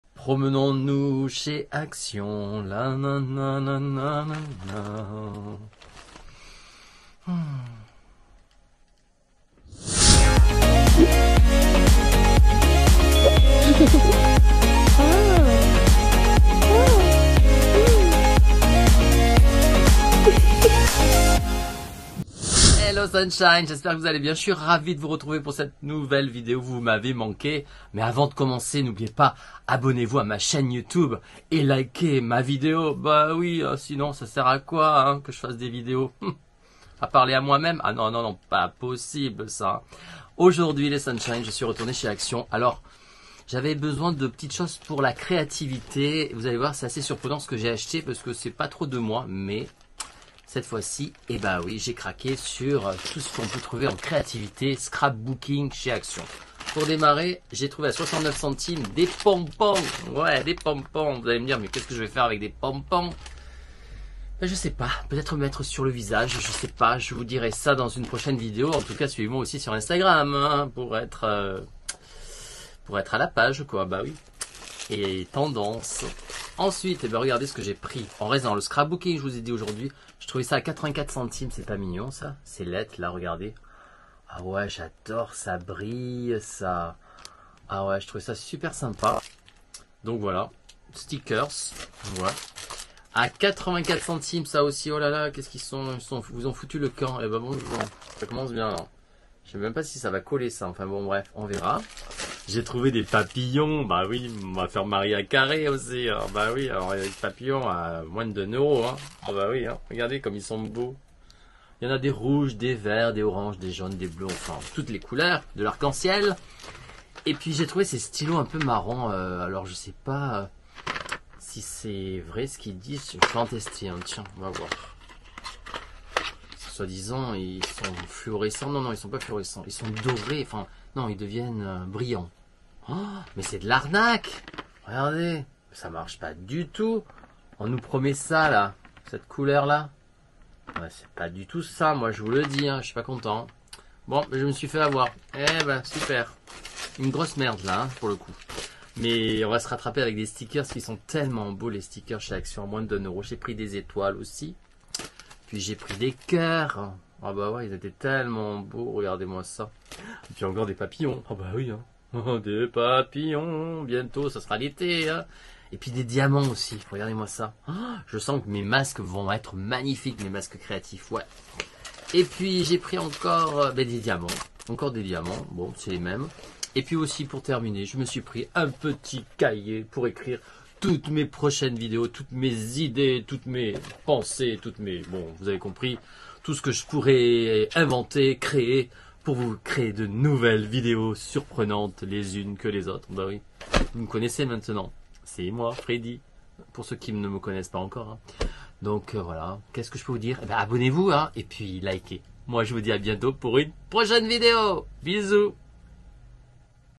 HAUL action scrap / que des nouveautés créatives ✌🏻
hello sunshines

c'est freddy et je te partage mes achats du jour

toujours des trouvailles chez action. On m'appelle action man souvent😂😂😂.

ce magasin renferme des petites scrap, beauté, gourmandises bref y'a de tout et à petits prix .